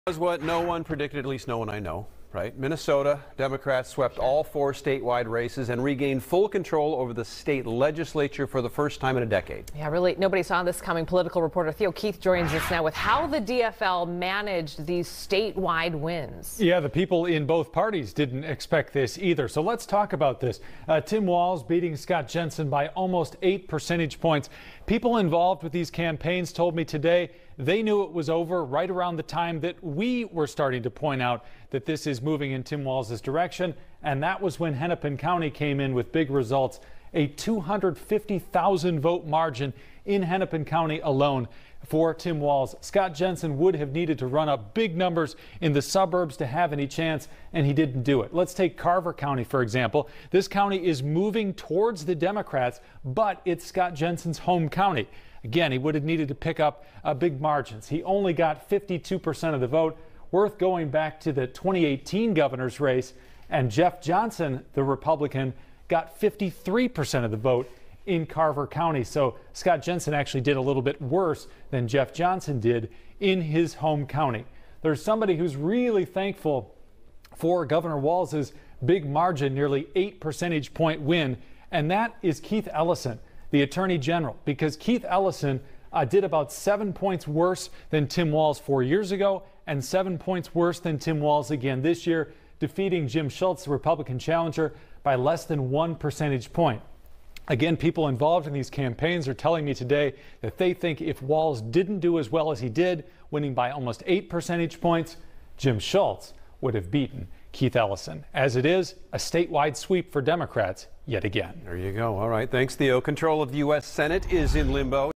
How the DFL swept on election night in Minnesota
Theo Keith breaks down how the DFL swept on election night in Minnesota, holding down major statewide offices, taking full control of the state legislature, and held down a tight and expensive race in Congressional District 2.